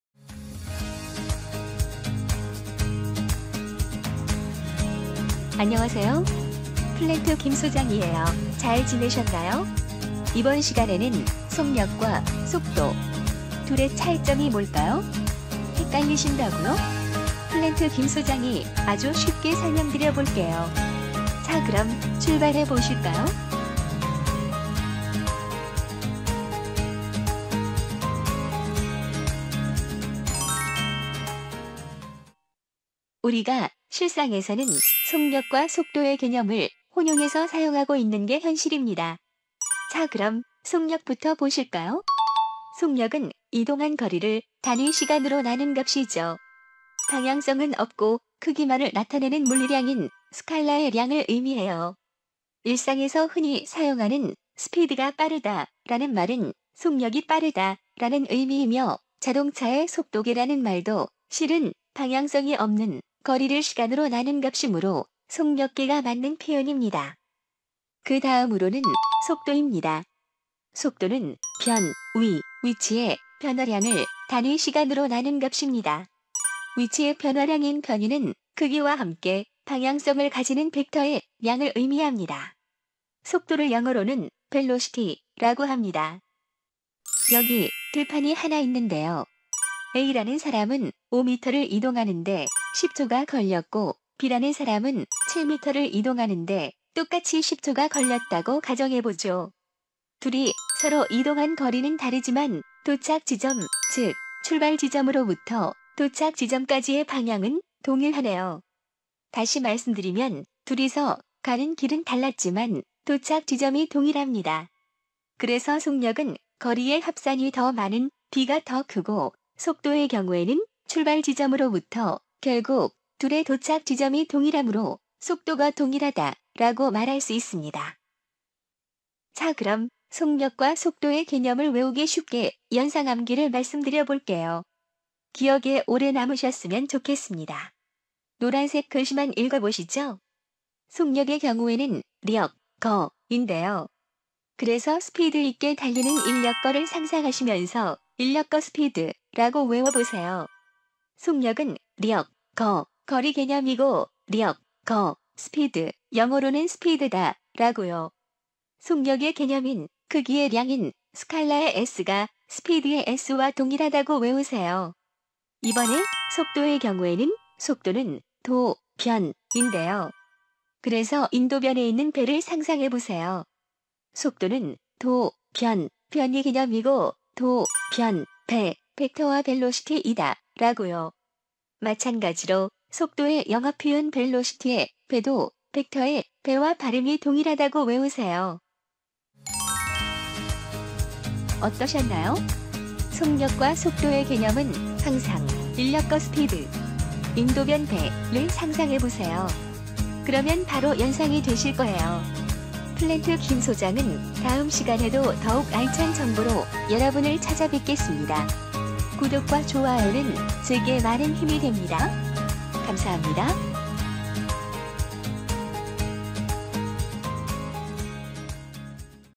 🧭 속력 vs 속도? 수능은 이 차이만 알아도 됩니다! 속력과 속도 완벽 비교! 수능 물리 개념 한방에 정리 (연상 암기법 포함)
안녕하세요! 수험생 여러분 👋
이번 영상은 수능 물리에서 자주 출제되는 개념,
바로 “속력과 속도”의 차이점을 쉽게 정리해드립니다.

📌 영상 구성 요약
🌀 속력(Speed): 방향 없는 스칼라 양
➡️ 전체 거리 / 시간

➡️ 예시: 자동차가 왕복 100km 달려도 속력은 거리 중심!

🧭 속도(Velocity): 방향을 포함하는 벡터 양
➡️ 변위 / 시간

➡️ 예시: 같은 100km를 왔다 갔다 하면 속도는 0?!

🔍 이 둘을 헷갈리지 않는 연상 암기법 소개

속도: 방향 있다 → "도"는 방향 잡는 '도(導)'

속력: 거리만 계산 → "력"은 순수한 '힘(力)'

📘 수능 기출문제에서 속력/속도 구분 포인트까지 포함

중·고등학교 물리 개념 헷갈리시는 분

수능 물리 개념 잡고 싶은 수험생

과학을 처음 배우는 중학생

물리 기초를 체계적으로 정리하고 싶은 분